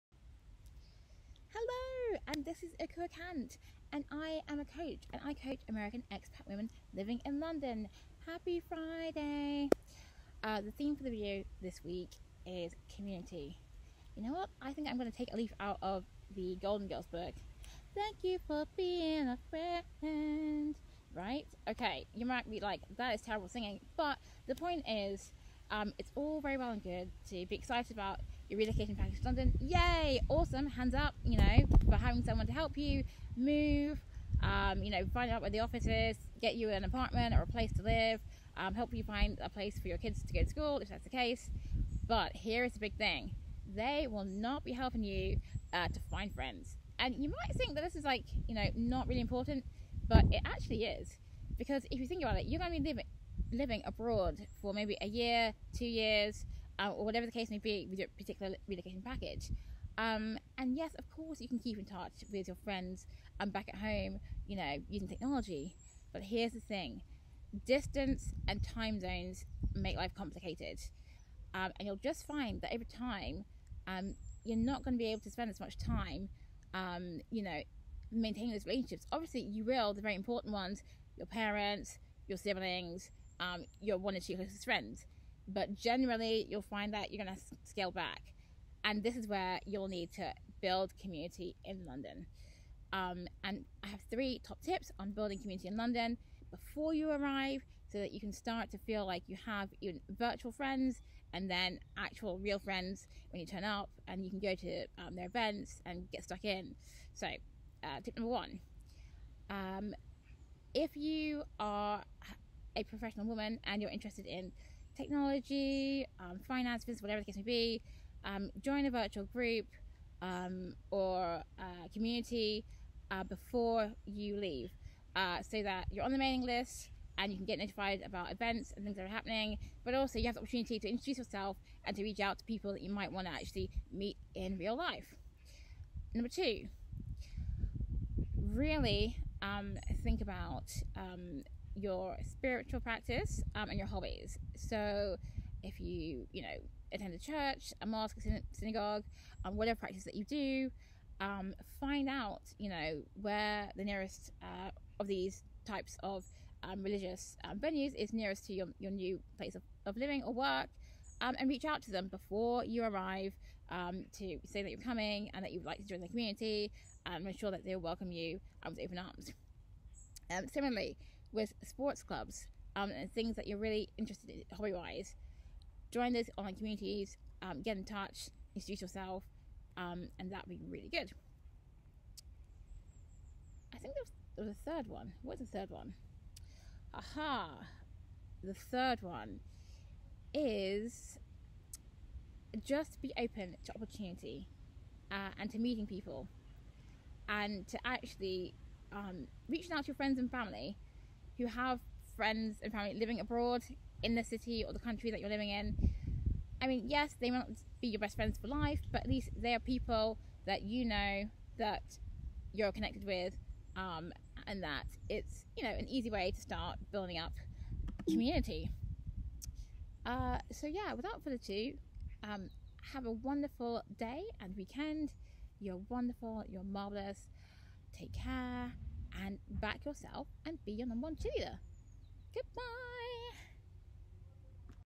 Be Your No 1 Cheerleader on Building a Community in London
Hello,

This video is about building your community before you move to London! We all want to be social and make new friends. In the video I give 3 tips for starting to build your community.

Warm Regards,

Ekua 💛